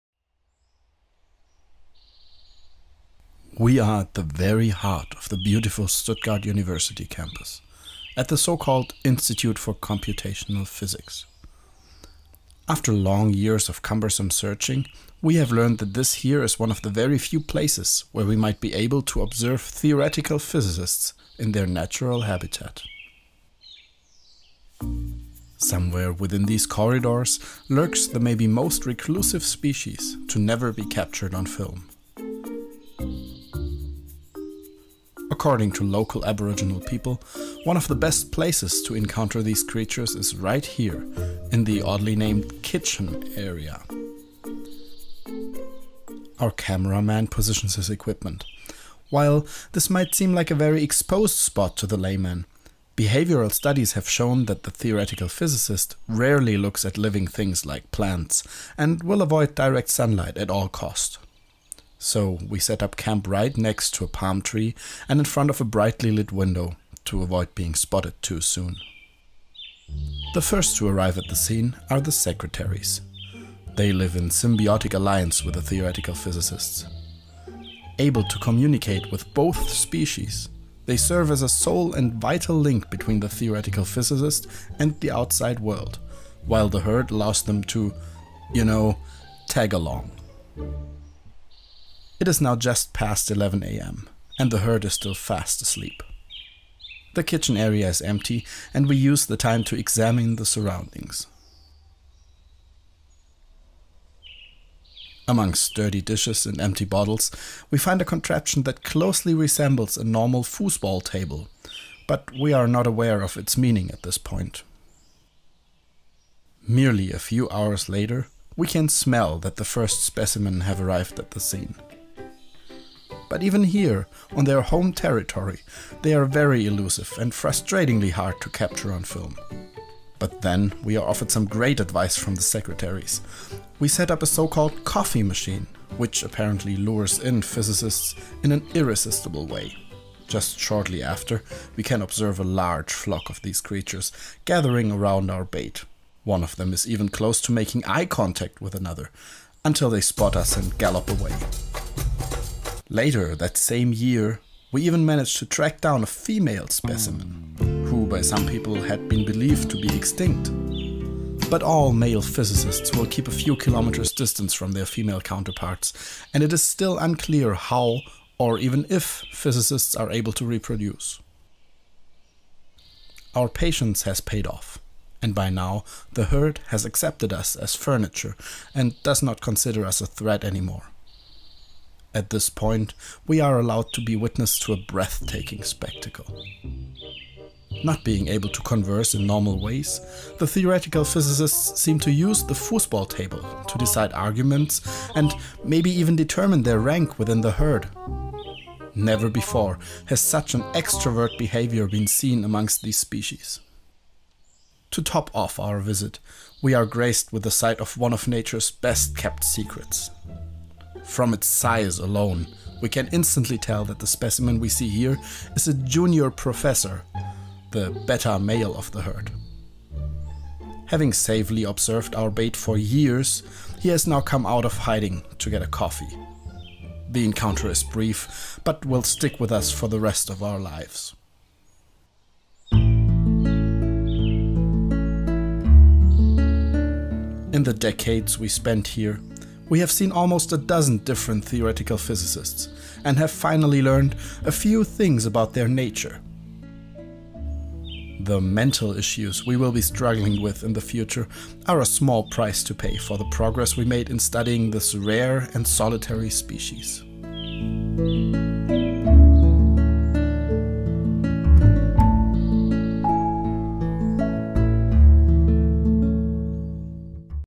Theoretical Physicists In The Wild
I made this video as a joke for the Christmas party of my workgroup. I restricted myself to one hour of shooting and one hour of post production, since I have a lot on my plate right now...So I myself see tons of things in there that I might improve upon if given a little more time. Don't judge too harshly. :)